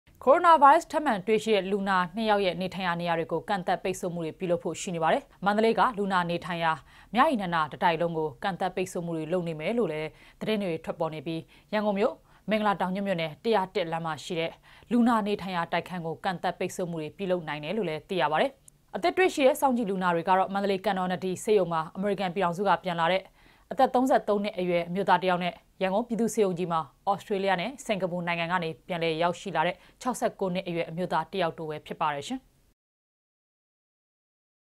DVB - ျမန္မာနိုင္ငံမွာ ကိုရိုနာဗိုင္းရပ္စ္ COVID-19 လူနာအသစ္ (၂)ေယာက္ ထပ္မံေတြ႕ရွိ
ကိုရိုနာဗိုင်းရပ်ထပ်မံတွေ့ရှိတဲ့ လူနာနှစ်ဦးရဲ့ နေထိုင်ရာနေရာတွေကို ကန့်သတ်ပိတ်ဆို့မှုတွေ ပြုလုပ်ဖို့ ရှိနေတယ်လို့ သိရပါတယ်။ မန္ဆလေးက လူနာနေထိုင်ရာ မြရည်နန္ဒာတစ်တိုက်လုံးကို ကန့်သတ်ပိတ်ဆို့မှုတွေလုပ်မယ်လို့ သတင်းတွေ ထွက်ပေါ်နေပြီး ရန်ကုန်မြို့ မင်္ဂလာတောင်ညွန့်မြို့နယ် ၁၀၁ လမ်းမှာရှိတဲ့ လူနာနေထိုင်ရာ တိုက်ခန်းကို ကန့်သတ်ပိတ်ဆို့မှုတွေ ပြုလုပ်နိုင်တယ်လို့ သိရပါတယ်။